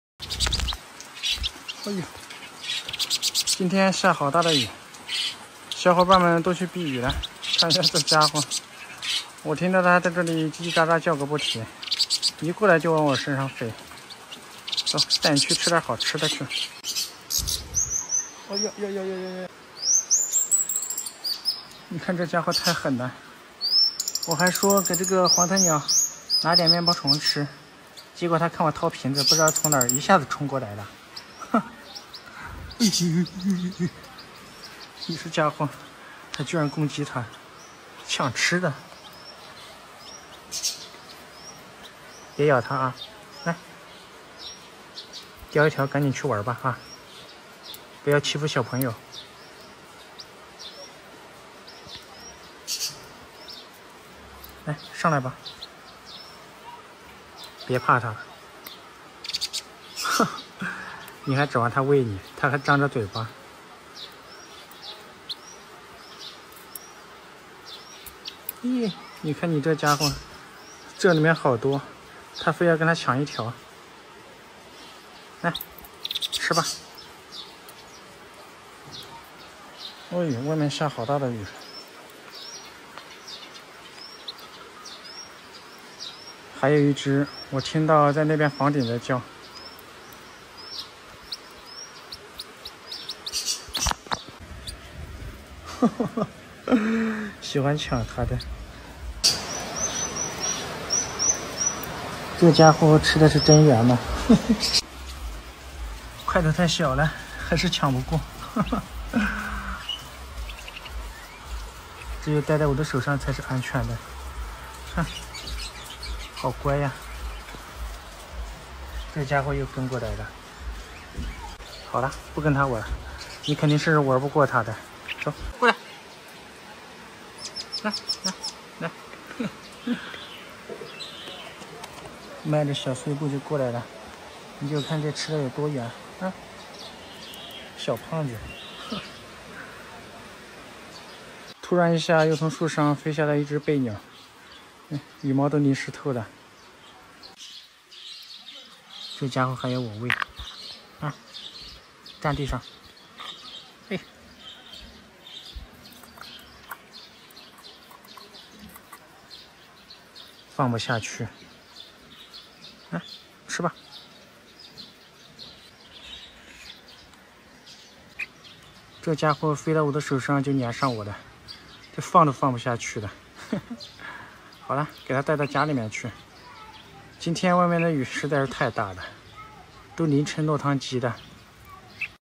兩隻小鳥針鋒相對，只為小夥手上的自助大餐，一口一條超過癮【蒙面鳥哥】
”视频内容已经过认真筛选，以保证其中没有任何形式的动物虐待。”
哈啰大家！我是蒙面鸟哥！